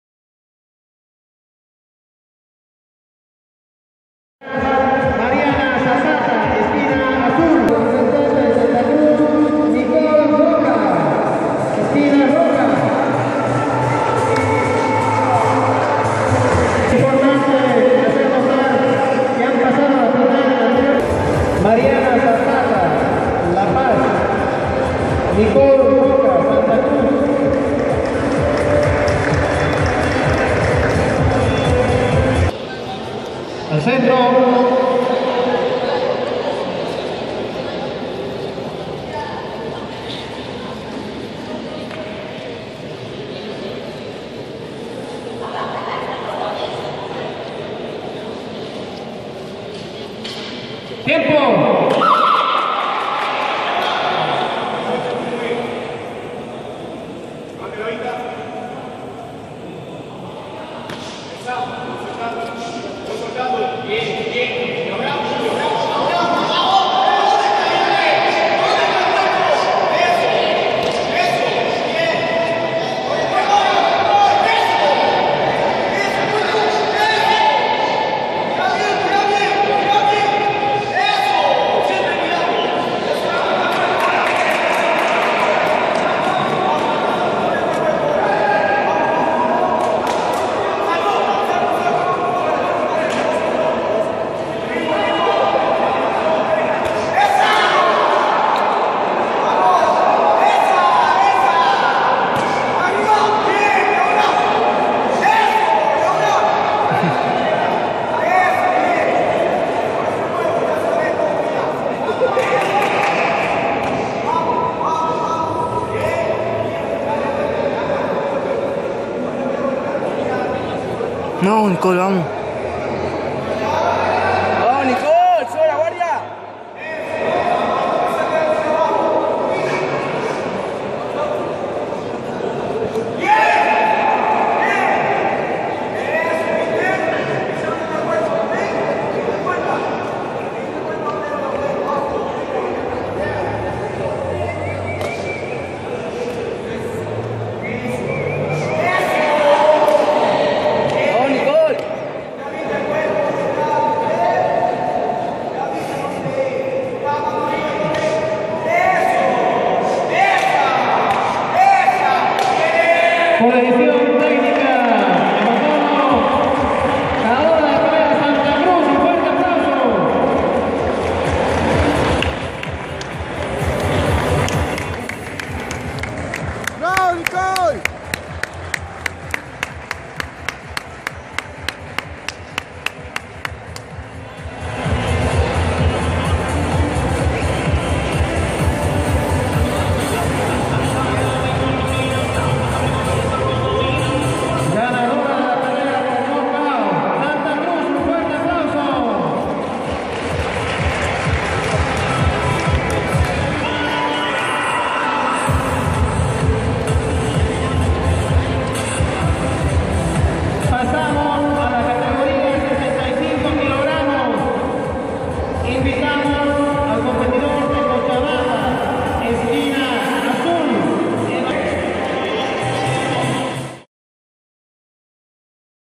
NÍCOL ROCA VS MARIANA ZAPATA NACIONAL TARIJA 2015
HOLA QUERIDOS AMIGOS QUI LES DEJO ESTE VÍDEO DE EL PRIMER NACIONAL DE NUESTRA TRI CAMPEONA NACIONAL NÍCOL ROCA LAVAYEN ESPERO LES GUSTE BENDICIONES OOSS.